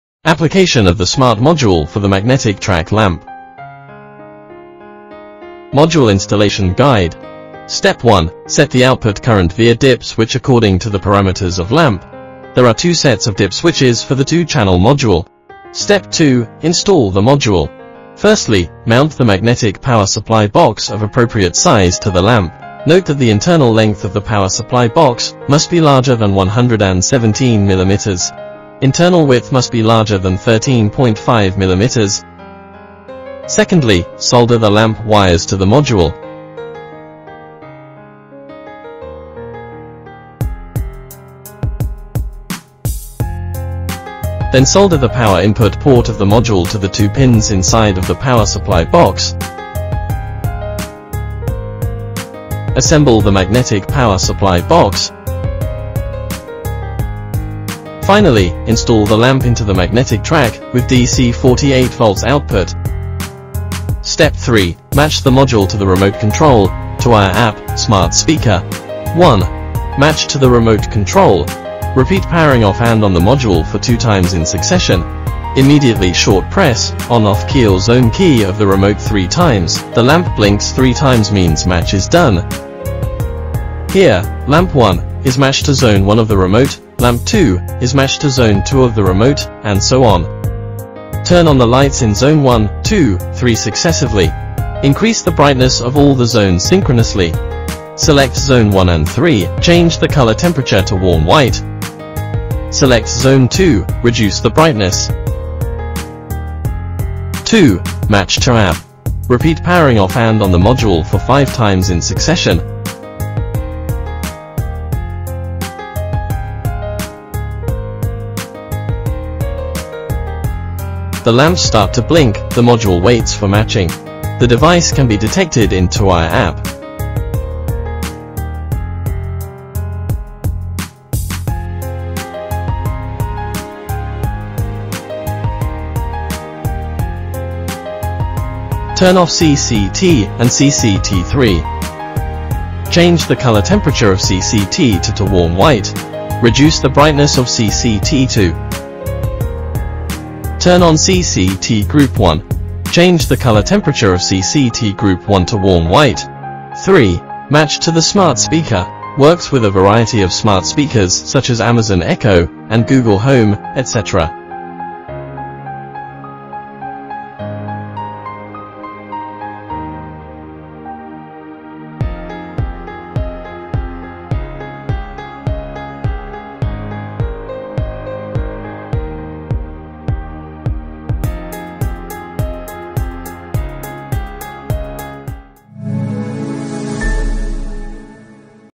How to install and use Tuya Smart LED Dimming Drivers for Magnetic Track Lamp
SKYDANCE Tuya smart LED dimming drivers specially for  Magnetic Track Lamp , support Tuya app control , Voice control, remote control .